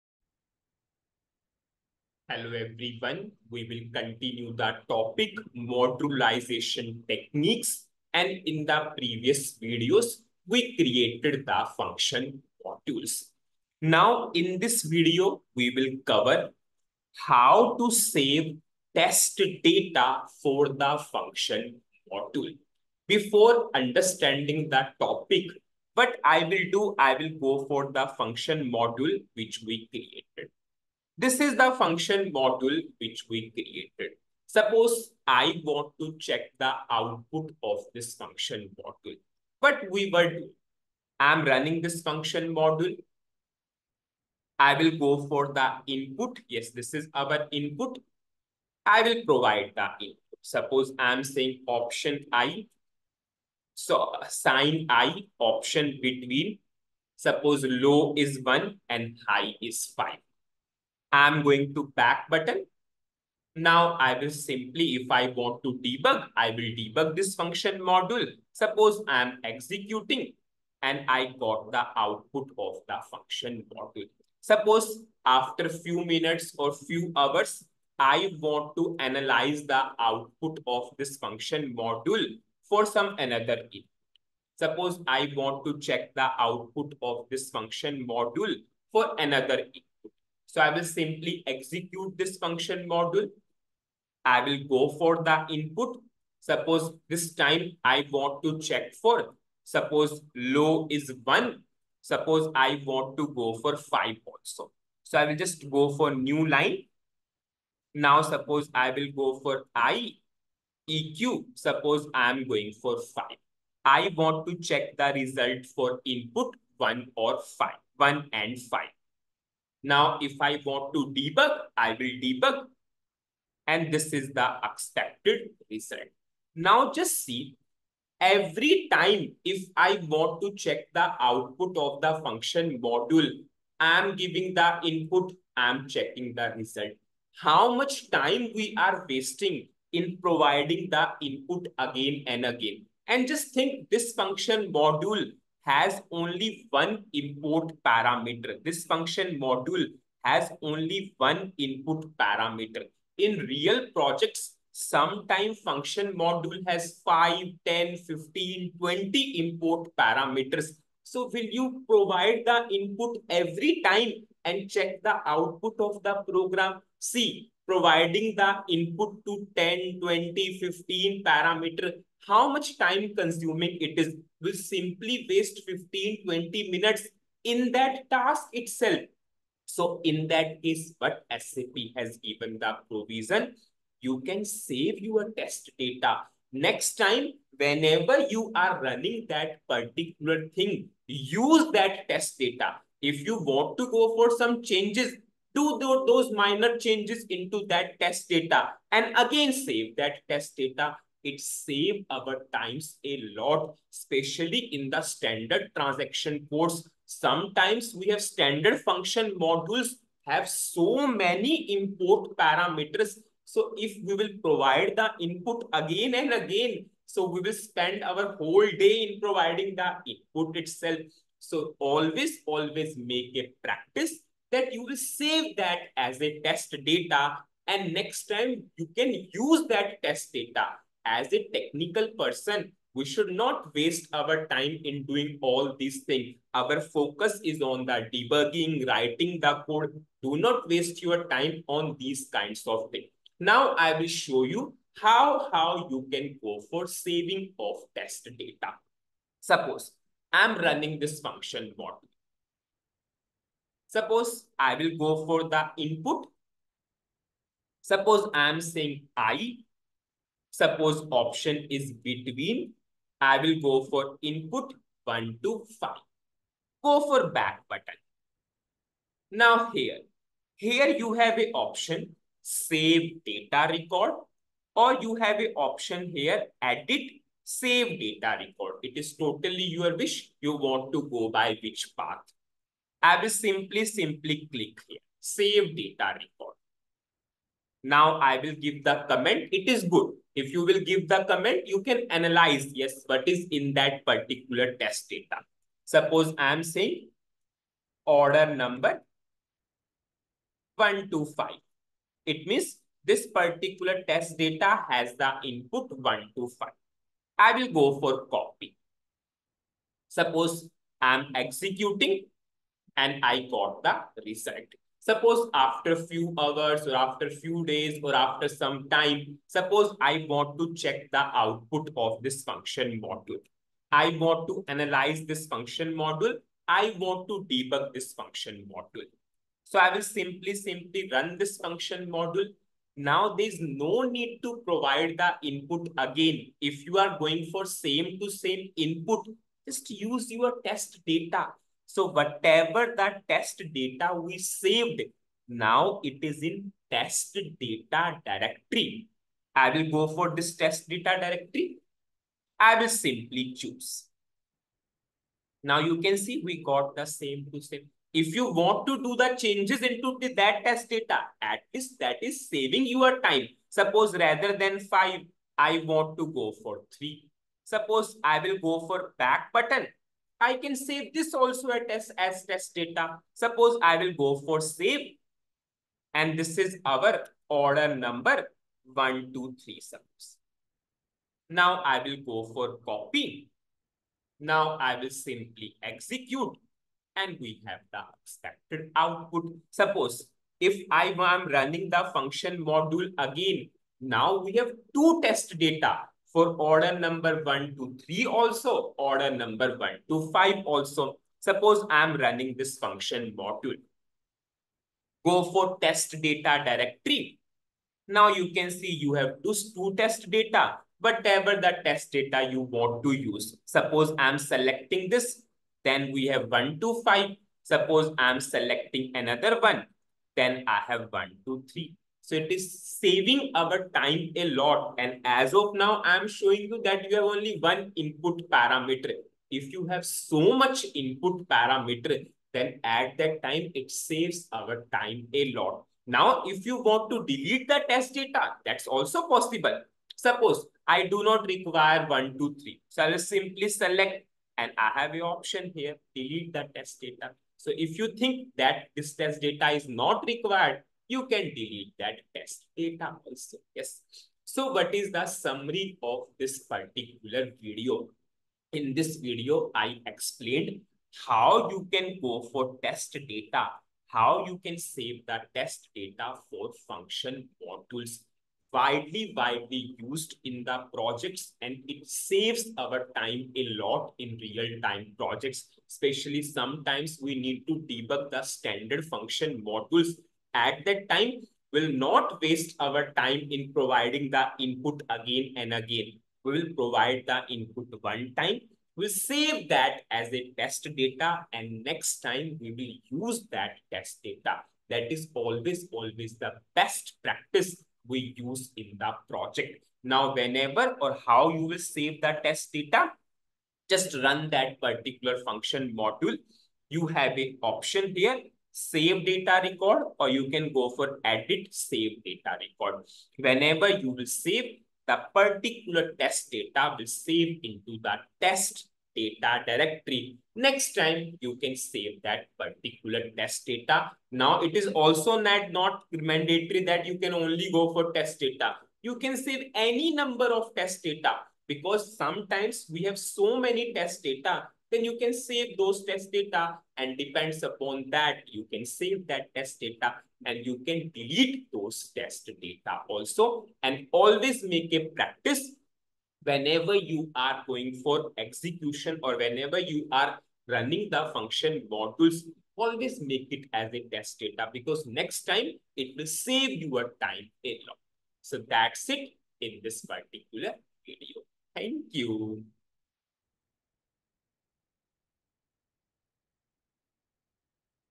22 - Modularization Techniques - Function Modules - Save Parameters as Test Data
-Practical Demonstration of Saving the Function Module Parameters as Test Data.